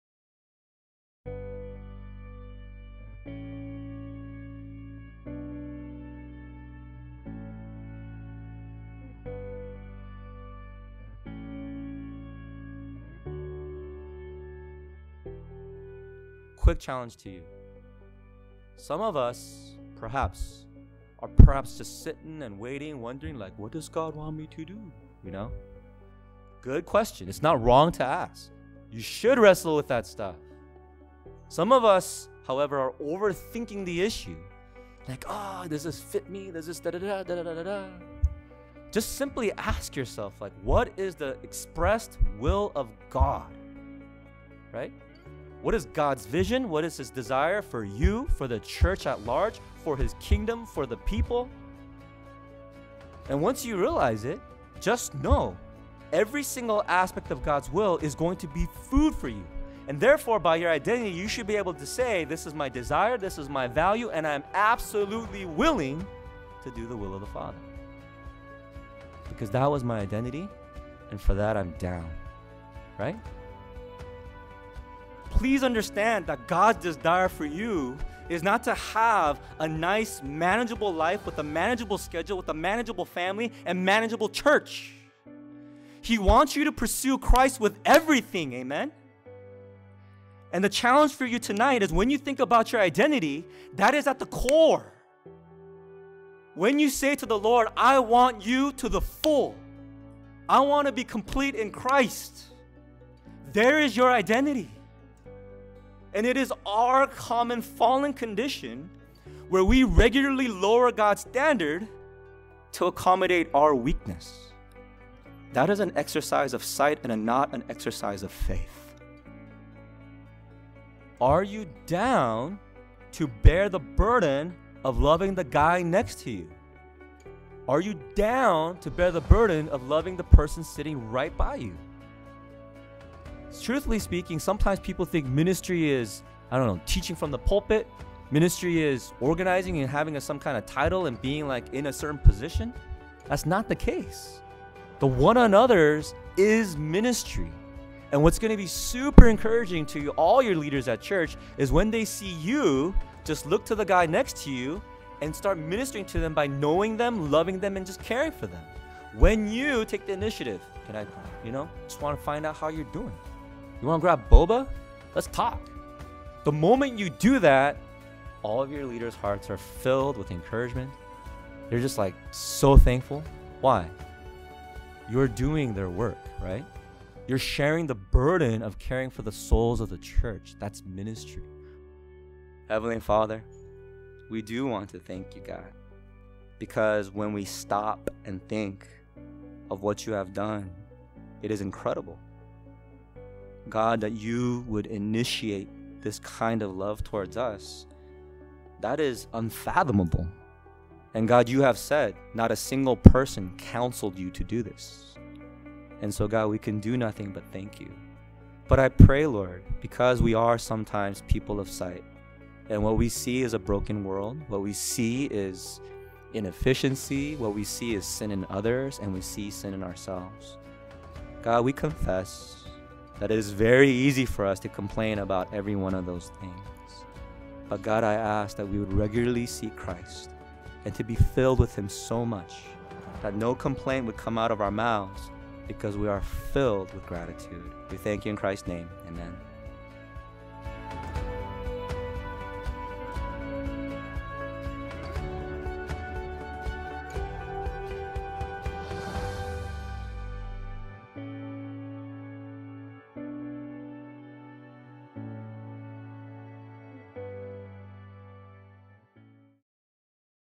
FCBC Walnut In-Transit 2019 Summer Retreat
FCBC Walnut's young adult ministry (In-Transit) had their biennial retreat at Murrieta Hot Springs with the central them on Identity in Christ.  The speaker of the retreat was Mark Lim, who is currently the Executive Director and Youth Pastor at Berean Community Church in Irvine.  Footage and film was capture using a Zhiyun Smooth 4.